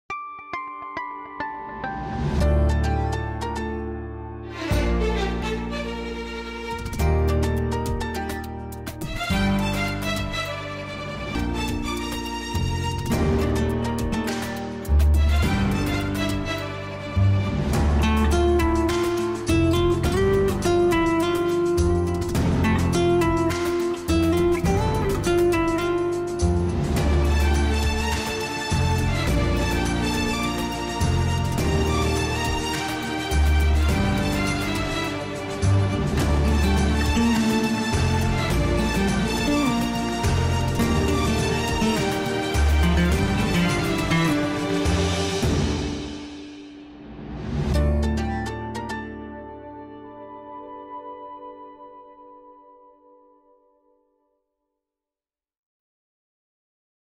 Magma Housing offers "Credit Linked Subsidy Scheme" under PMAY.
The "Credit Linked Subsidy Scheme” (CLSS) under Pradhan Mantri Awas Yojana (PMAY) was announced with the vision of housing for all by the year 2022. Magma Housing offers "Credit Linked Subsidy Scheme" under PMAY. Our PMAY Champions are ready to help our Customers to avail subsidy up to Rs. 2.67* lacs